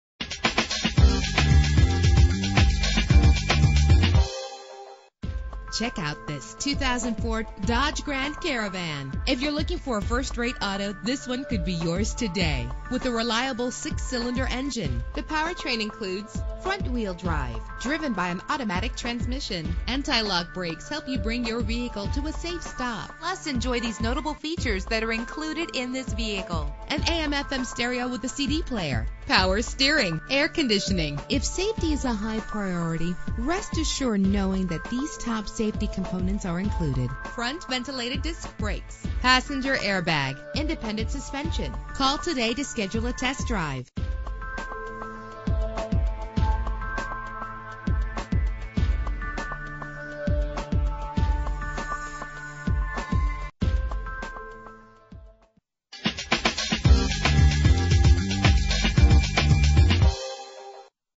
2004 Dodge Grand Caravan - Ocala FL
Year: 2004
Make: Dodge
Model: Grand Caravan
Trim: SE
Engine: 3.3 liter 6 cylinder 12 valve
Transmission: 4-Speed Automatic
Color: Silver
Mileage: 68958
Address:
3440 South Pine Avenue